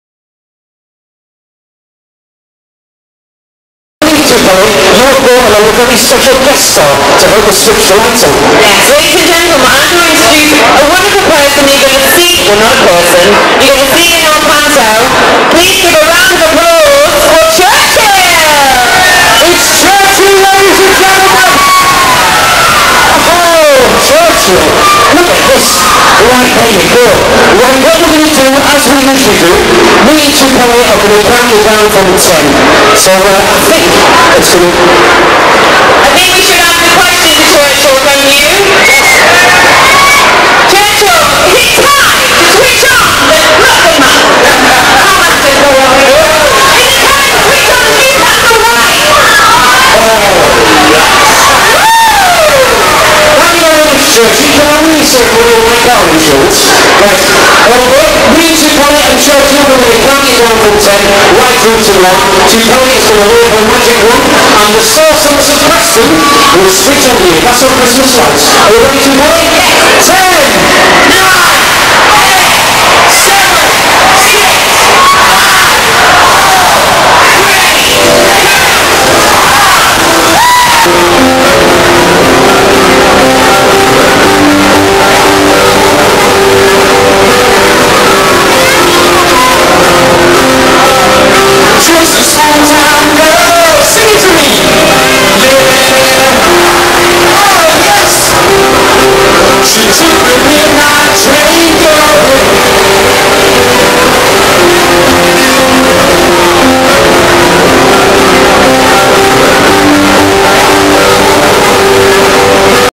Actress turns on Newcastle's Christmas lights
Families flocked to Grey Street to watch the lights go on and officially mark the start of the festive season last night. And pantomime sorceress Tupele Dorgu was on hand to spread some fairytale magic over the event.